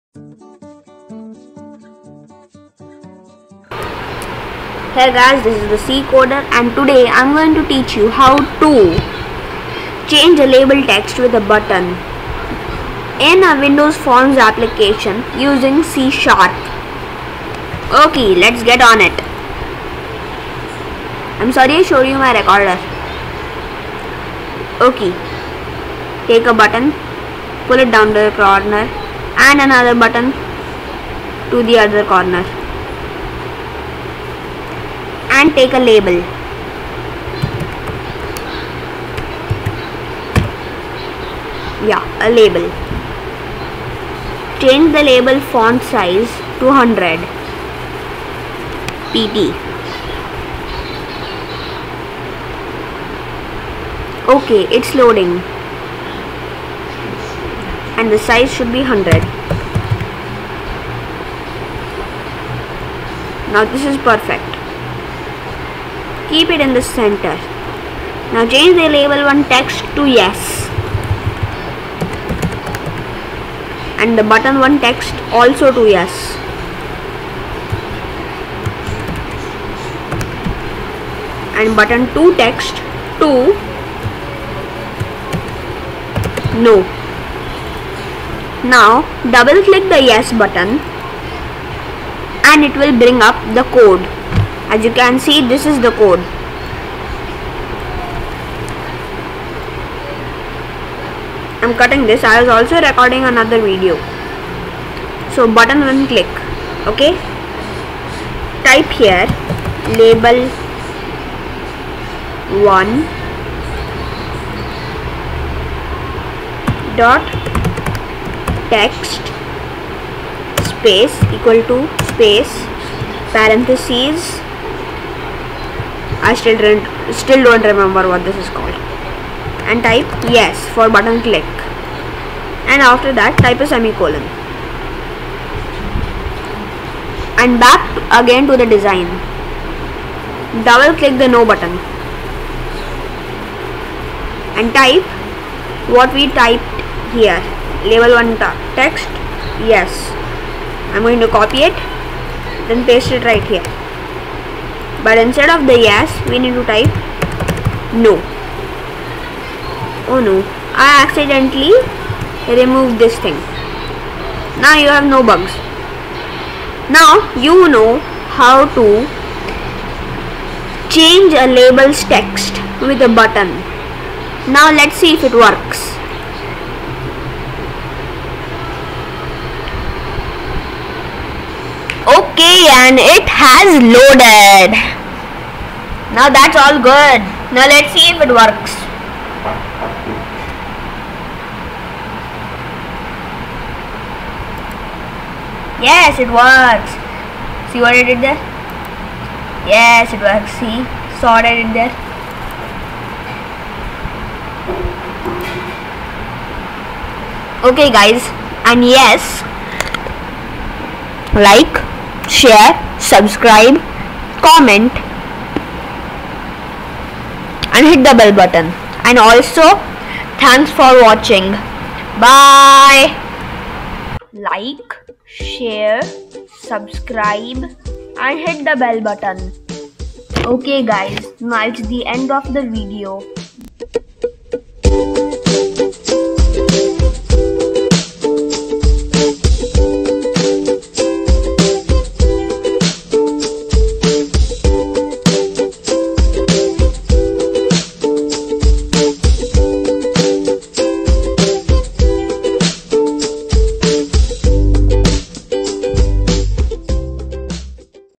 HOW TO CHANGE LABEL'S TEXT USING A BUTTON WITH C# - EPISODE 5 - THE C CODER
IN THIS VIDEO I WILL TEACH YOU HOW TO CHANGE A LABEL'S TEXT USING C# IN A WINDOWS FORMS APPLICATIONS

0:00 START
0:23 UI
1:31 CODE
2:56 BUILD/TEST

FOR HELP AND PROBLEMS SEND AN EMAIL TO ONE OF THE ABOVE LINKS

License